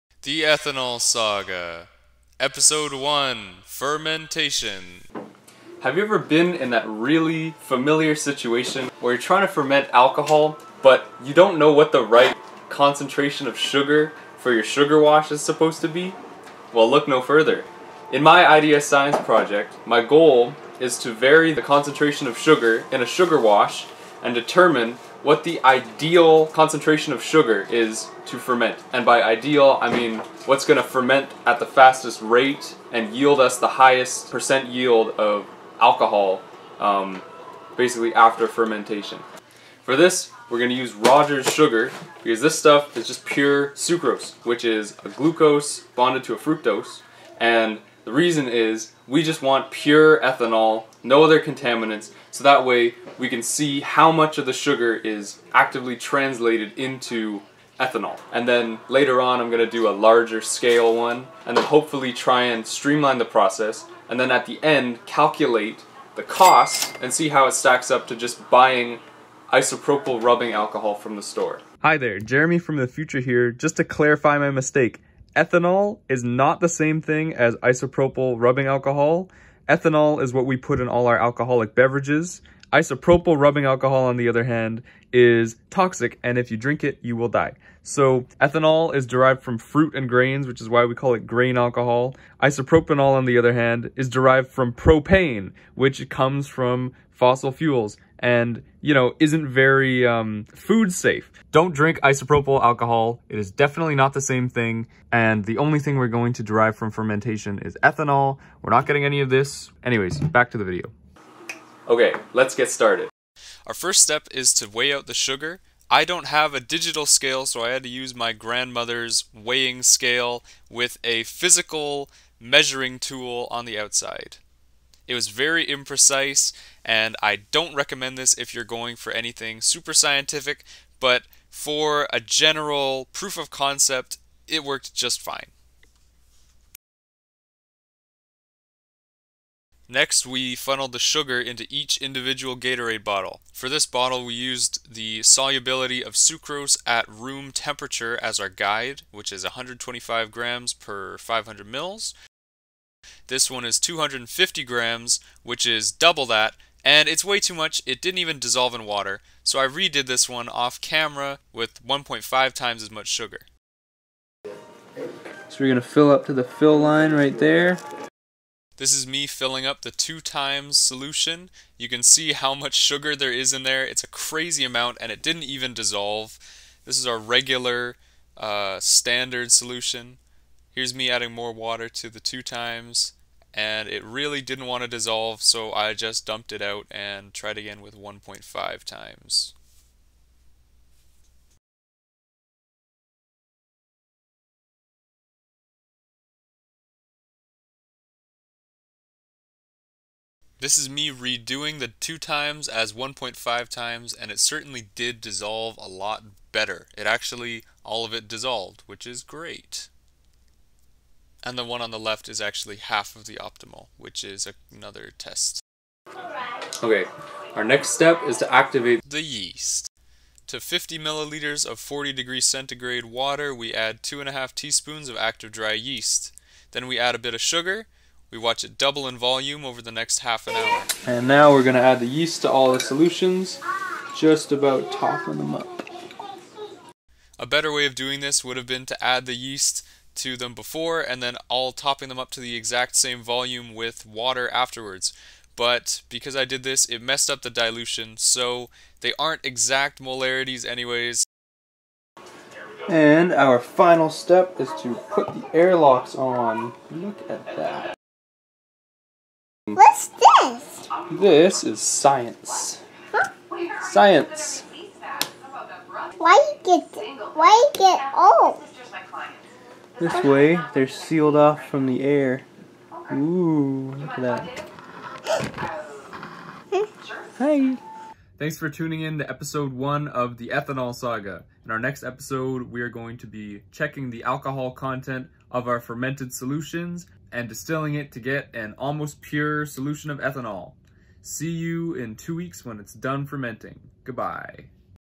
Sugar Wash Fermentation
Episode 1 of the Ethanol Saga.

In this video, I show how to prepare and ferment a sugar wash, a mixture of just water, yeast, and sugar.

By the end, I will make fully-functional hand sanitiser